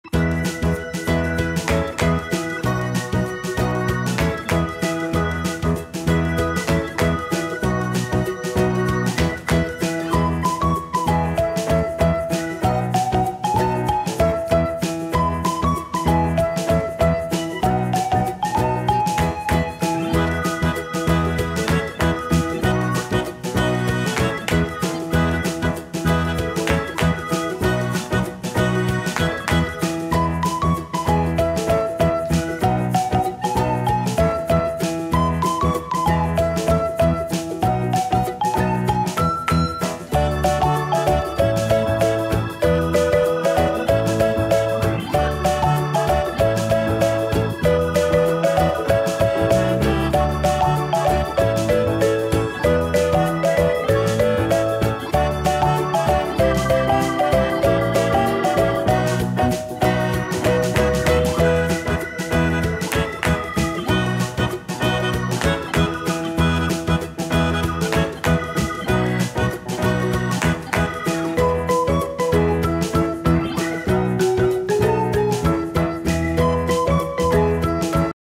😭 Animoji Real meanings 2019 🤯💩🤧
💩 THE POOP EMOJI ON AN ICE CREAM CONE or piece of shit?  Animojis explained: What are the real meaning of emojis that we use every day? hidden meanings of emojis. texting tips of 2019.
how to flirt with emojis.